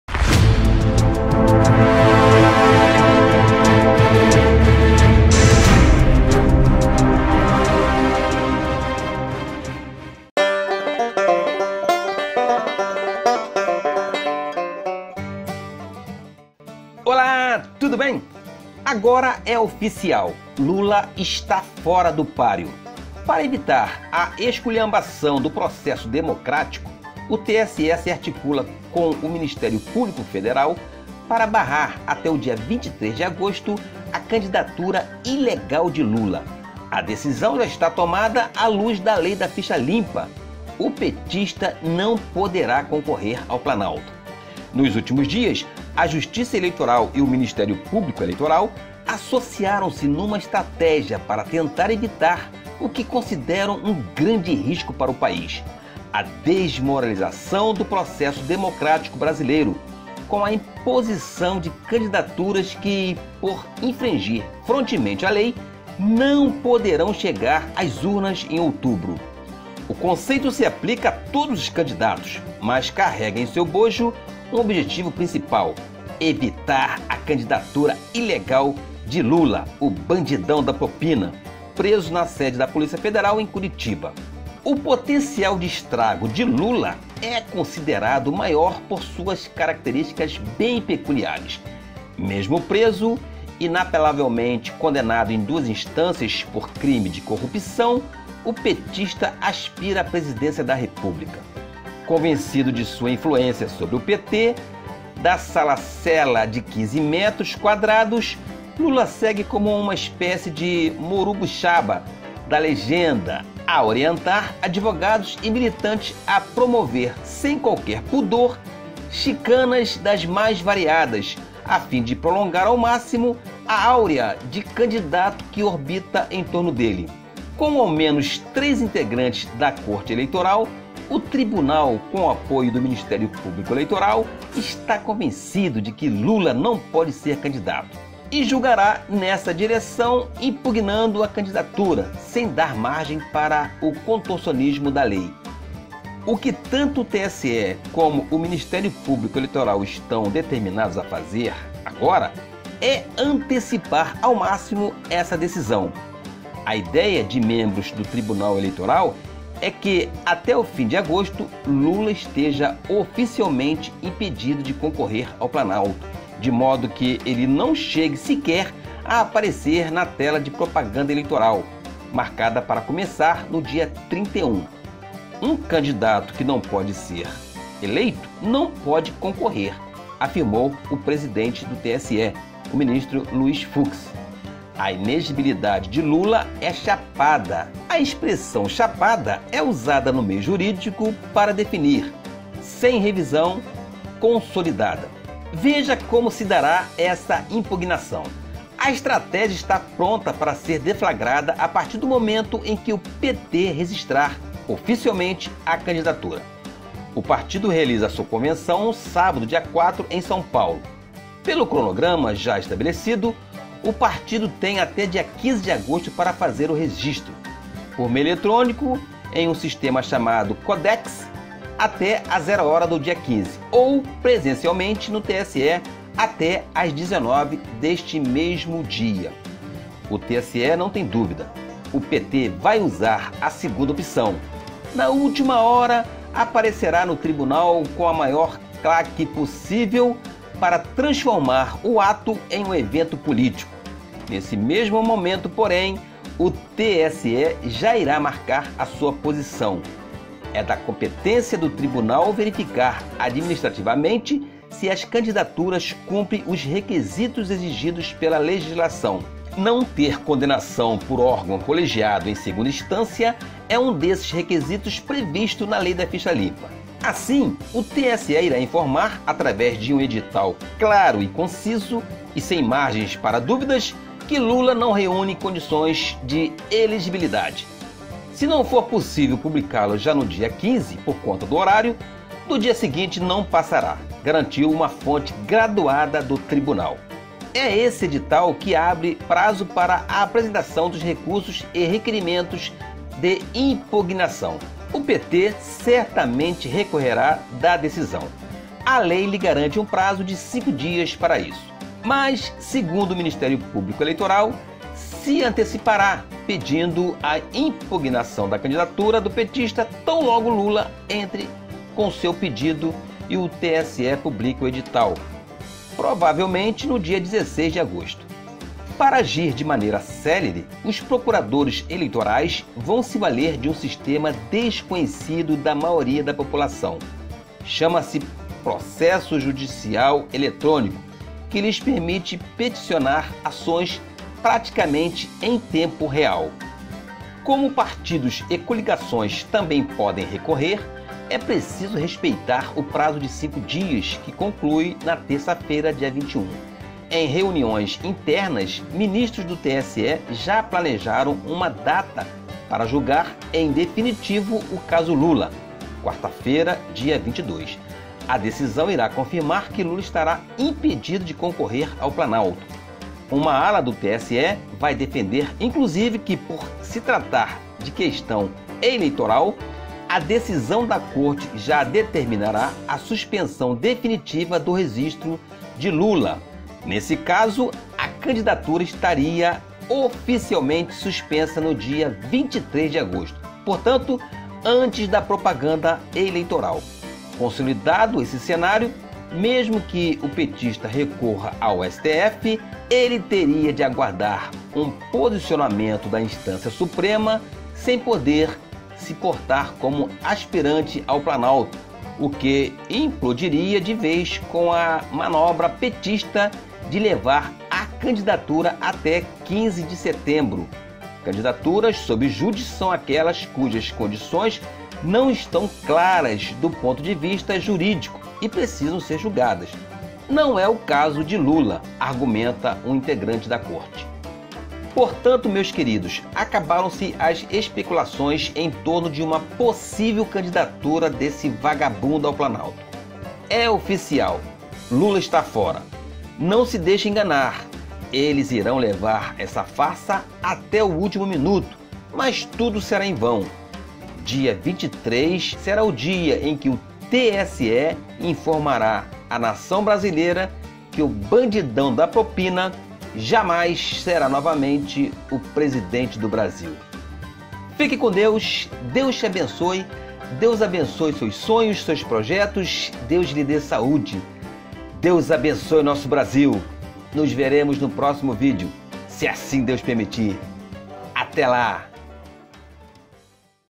AGORA É OFICIAL! TSE E MP ELEITORAL AFIRMAM: LULA ESTÁ FORA DO PÁREO DEFINITIVAMENTE.
LULA E PT TEMEM VAIAS E DESPREZO NO RIO PELO APOIO A CABRAL, GAROTINHO, ROSINHA E PEZÃO.

LULA DIZ QUE POLÍCIA DO RIO É BURRA E PRECISA SER DESARMADA - FIASCO TOTAL NA CARAVANA .

MALDITO LULA SE OPÕE AO PLANO REAL E DIZ QUE COLLOR E RENAN DÃO SUSTENTAÇÃO AO GOVERNO.

MP, PF, STF! PORQUE LULA AINDA NÃO ESTÁ NA CADEIA? ELE É O CHEFE DA QUADRILHA.